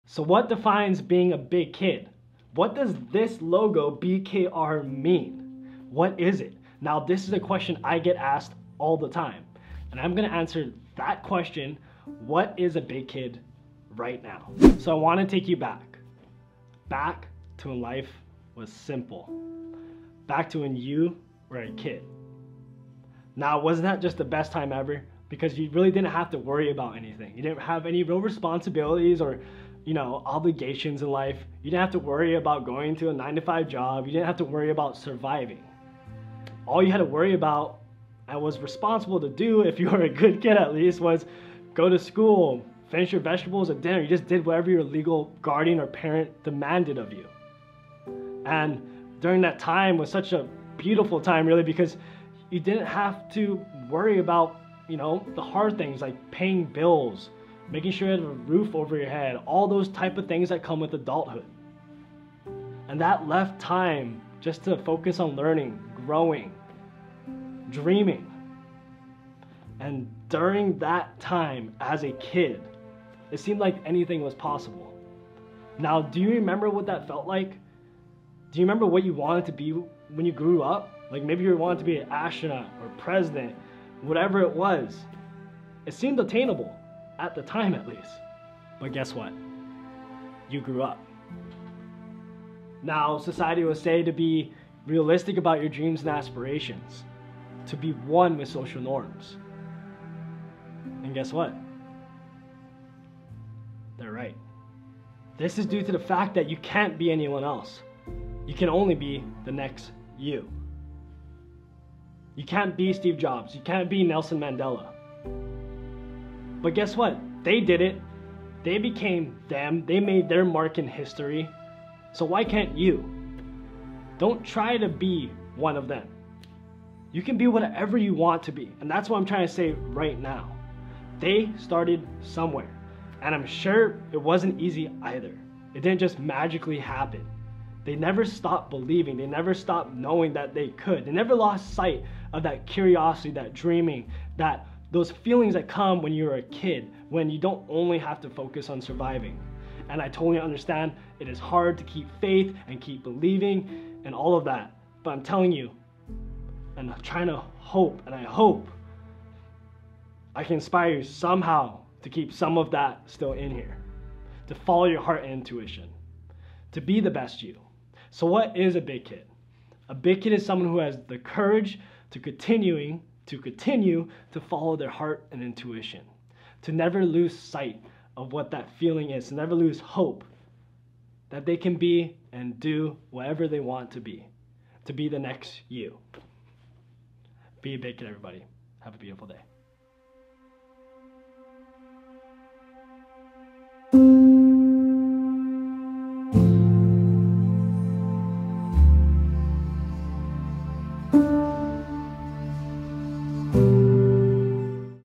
BKR | WHAT'S A BIG KID?!
Today I answer the question I get asked the most. What's a big kid? What does my logo stand for? What is my brand? WHAT IS BKR?

SO WHAT DEFINES BEING A "BIG KID." THIS IS A QUESTION I GET ALL THE TIME. I'M GOING TO ANSWER THAT QUESTION FOR YOU RIGHT NOW.
I want to take you back to the days when life was simple. The days where your ONLY REAL responsibility was doing what your parent or legal guardian demanded of you. If they said to go to school or eat. You'd do just that.
No bills to pay, no obligations, no nothing.
ONLY GROWING. LEARNING. DREAMING.
Those were the days were anything seemed POSSIBLE.
Do you remember what you wanted to be when you grew up? What your dreams and aspirations were as a kid?
Maybe you remember that feeling... The burning desire to do something great. AND MAYBE you still have it.
YOU GREW UP.
Now SOCIETY will say to be REALISTIC about your goals and aspirations.
TO BE ONE WITH SOCIAL NORMS.
This is coming from people who GAVE UP on dreams. Who DECIDED they can't be the next STEVE JOBS or NELSON MANDELA.
THEY'RE RIGHT.
This is due to the fact, that you can ONLY be the next YOU.
You can't be anyone else.
All those people that did in FACT, make a MARK in HISTORY; started SOMEWHERE.
It didn't just MAGICALLY HAPPEN.
AND
I'm quite POSITIVE it wasn't EASY either.
If they did it, SO CAN YOU. You're still here, aren't you? Still living and breathing right?
You got time on your side. But don't take it for granted. Time will move forward regardless if you like it or not. Don't PROCRASTINATE your life.
The OPPORTUNITY to make YOUR mark in HISTORY is possible!
So what's a BIG KID you ask?
ANYONE who has the courage to follow their heart and intuition.
Ultimately, never letting that desire to live the life you want to live fade away with time.

△ Favorite Lens
△ Favorite Microphone